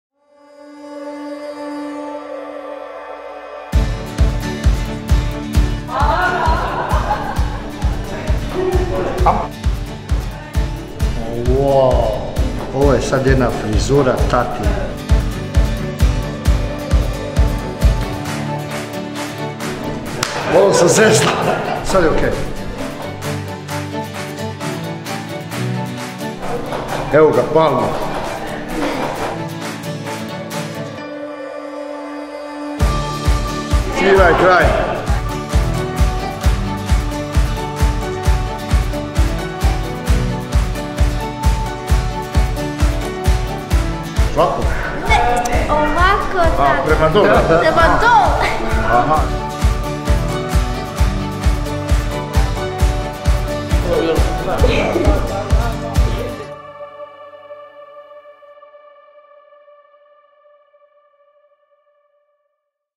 PANTENE – NOVO LICE SNAGE
I tata to može!

Mirza Džomba i Ivan Čupić u najslađem izazovu ikad

Proslavljeni hrvatski rukometni reprezentativci Mirza Džomba i Ivan Čupić zahtjevne sportske pripreme za Europsko rukometno prvenstvo – Hrvatska 2018. nakratko su zamijenili najslađim izazovom ikad.

Mirza Džomba i Ivan Čupić dio su kampanje pod nazivom „Novo lice snage“ jednog od najpopularnijih brendova za njegu kose. Cilj kampanje je potaknuti očeve da što više kvalitetnog vremena provode sa svojim kćerima. Istraživanja su pokazala da je kvalitetno provedeno vrijeme s očevima ključno u podizanju kćeri koje su samouvjerenije, samostalnije i uspješnije - kako u školi, tako i kasnije tijekom karijere.